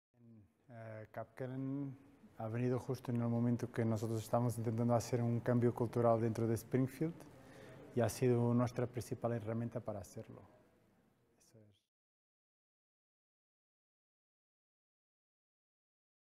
Cambio de CULTURA EMPRESARIAL en Springfield
Frederico Romao - Head of retail Operations nos cuenta la relación entre Springfield y CapKelenn y su cambio de cultura dentro de su empresa.
Descubre cómo realizar el cambio de cultura de tu empresa gracias a Capkelenn.